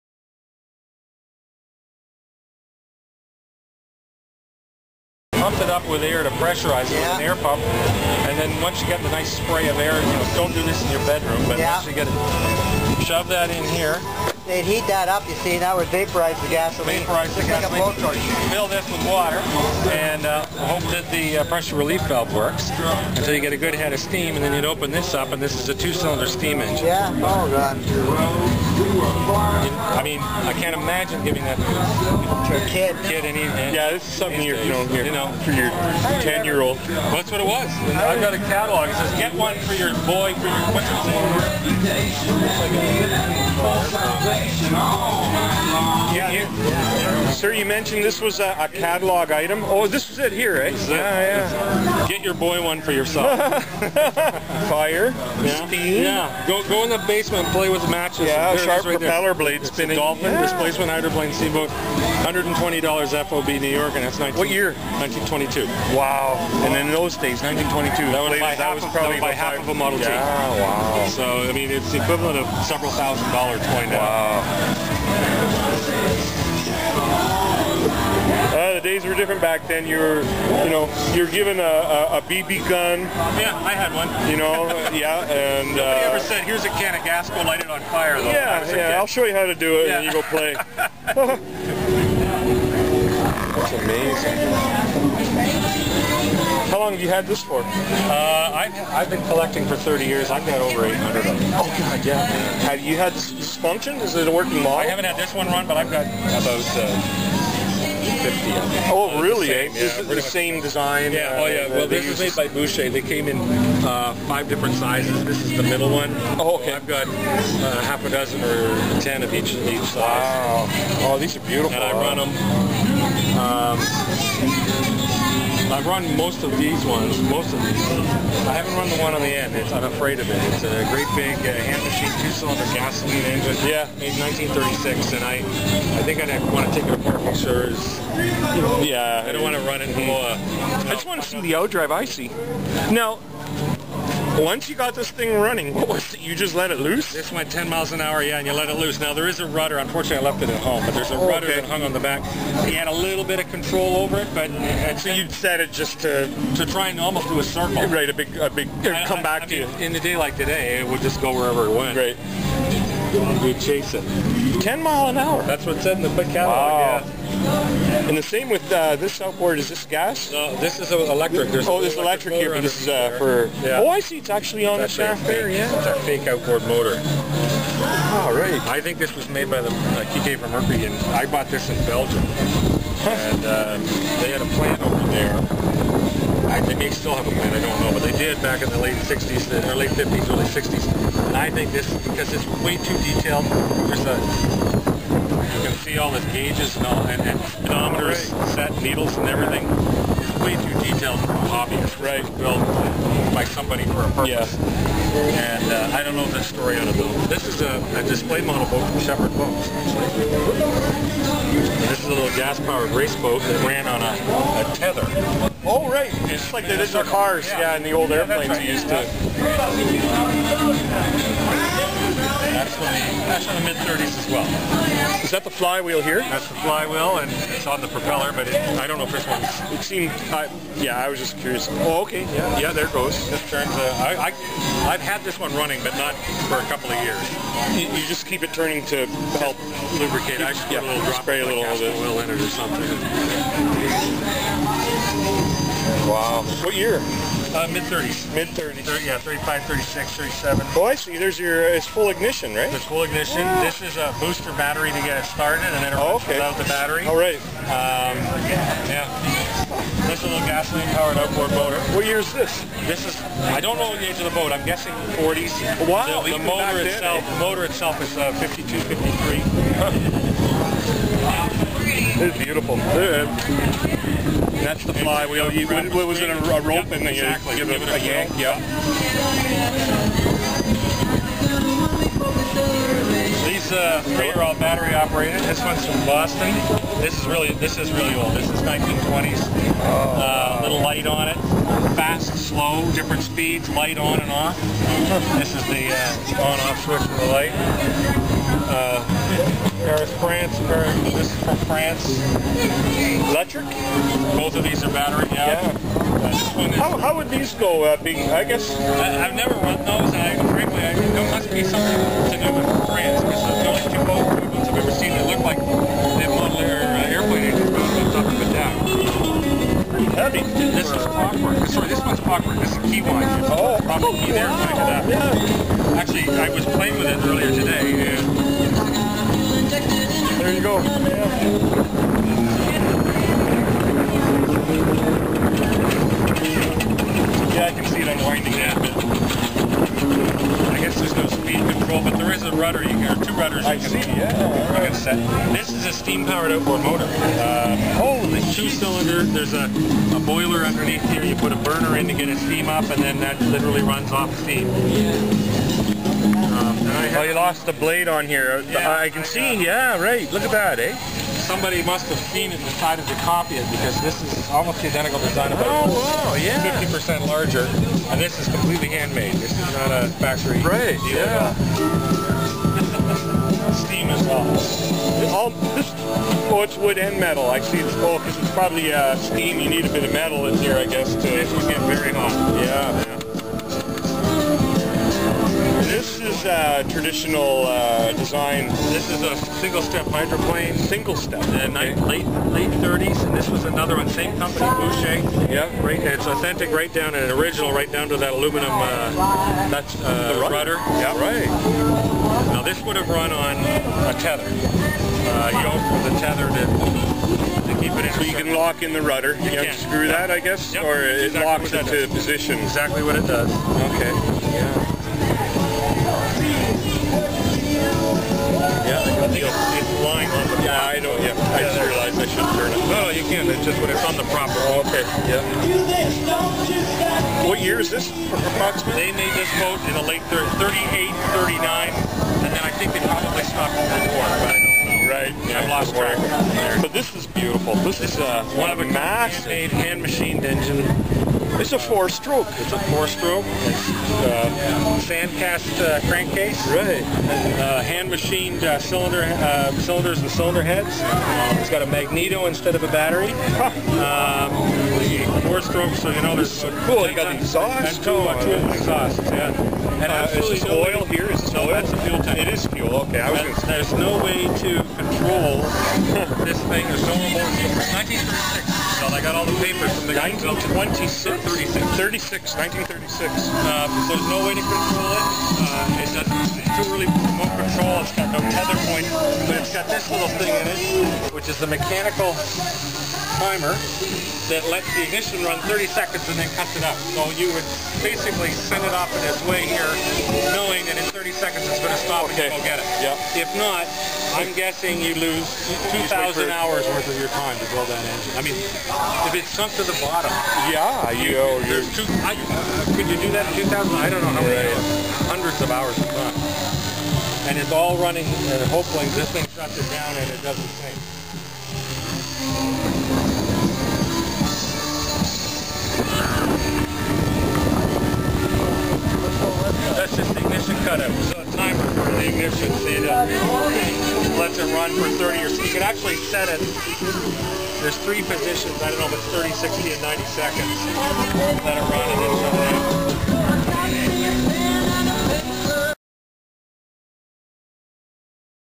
Rosseau Classic Cruise '09 Vintage Model Boats part 1
Vintage Gas Steam and Electric Model Boats on display at Rosseau Classic Cruise.
This is part 1 of 2 videos.